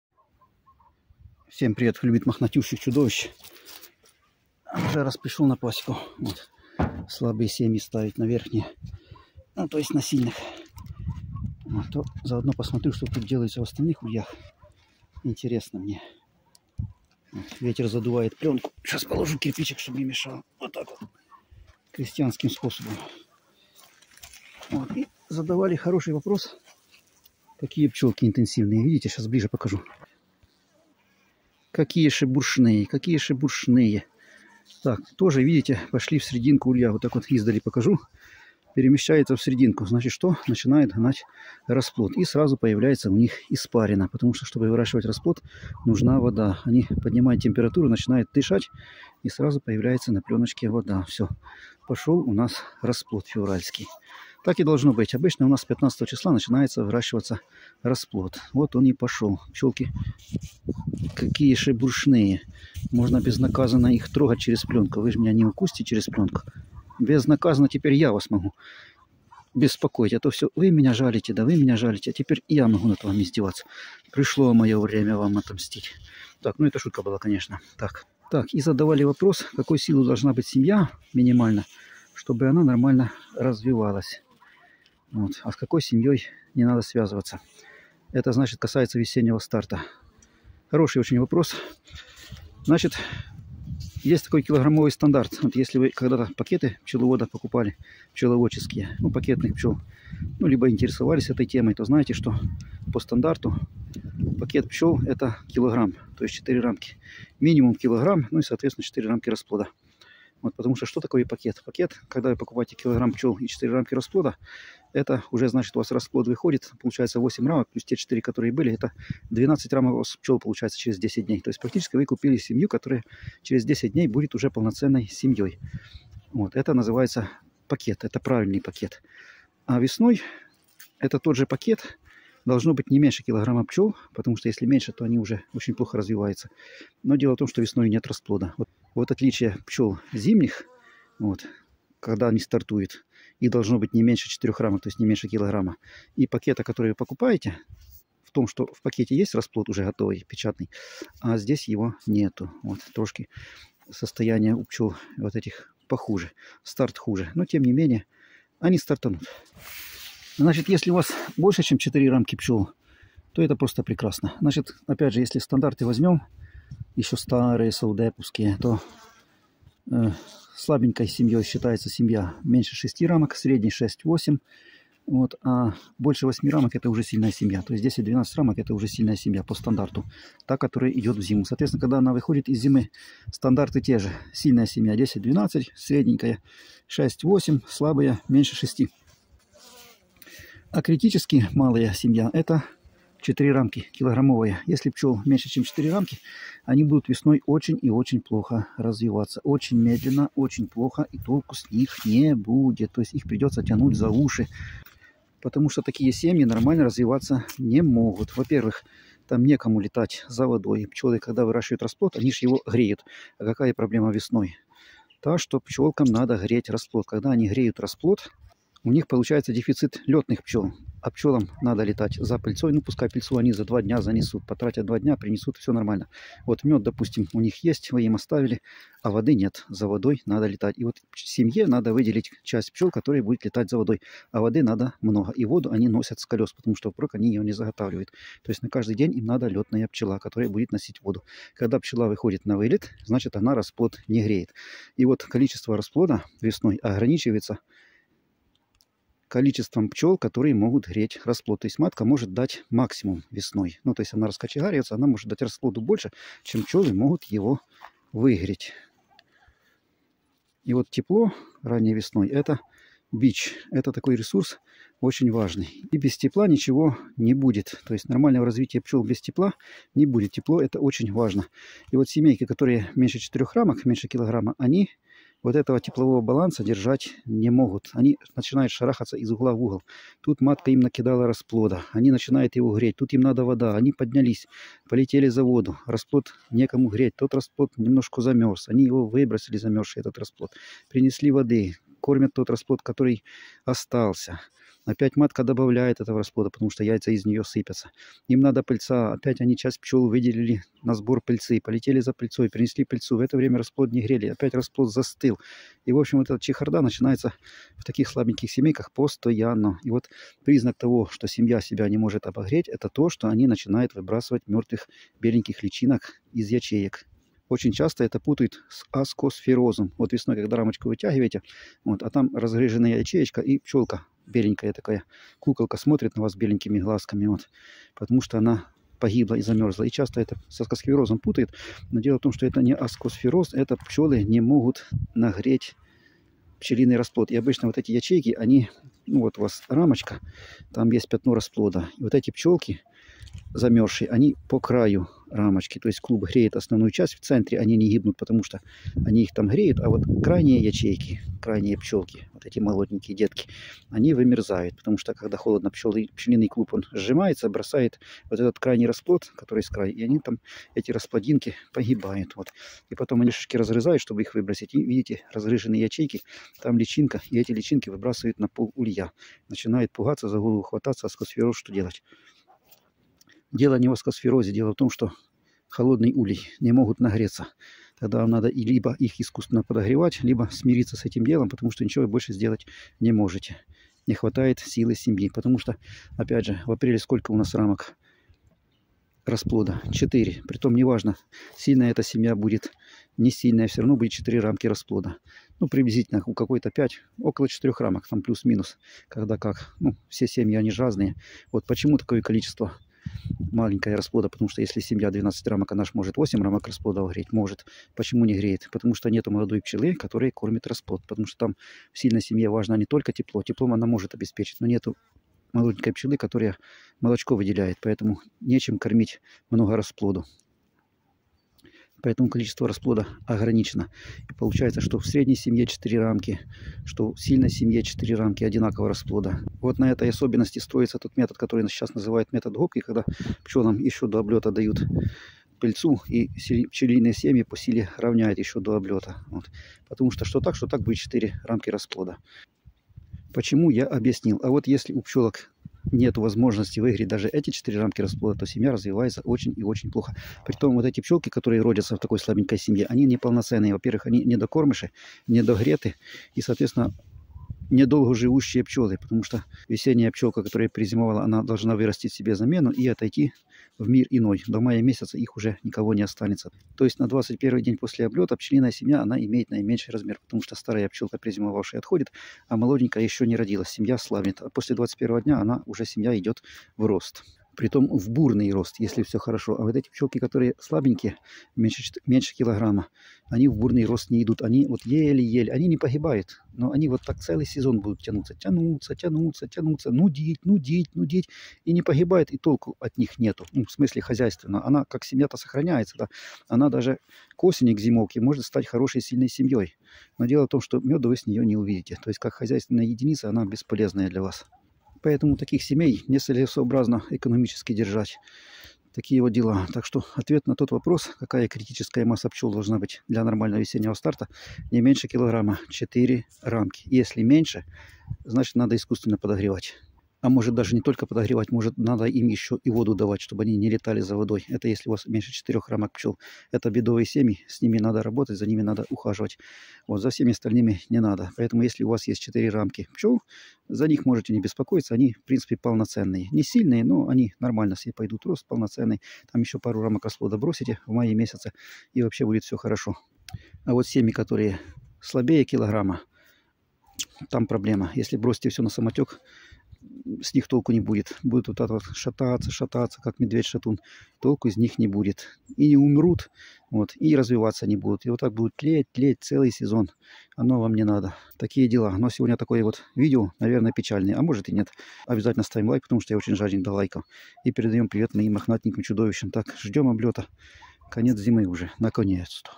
Пчёлы. 🐝 Какая минимальная семья должна быть для успешного старта весной? 🌱
Весна не за горами, и пчеловодам пора задуматься о готовности своих пчелиных семей! В этом видео мы расскажем:
✅ Какая минимальная масса семьи обеспечит успешное развитие весной.
✅ Почему важно учитывать численность пчёл после зимовки.
✅ Советы по укреплению слабых семей для эффективного старта сезона.

Не пропустите полезную информацию и практические советы! 📊🐝